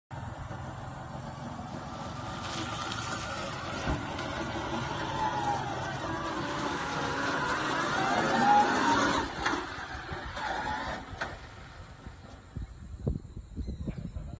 El mototaxi eléctrico para la región amazónica del Perú (Pucallpa) E TUK TUK
El mototaxi eléctrico para la región amazónica del Perú. Motocar , E Tuk Tuk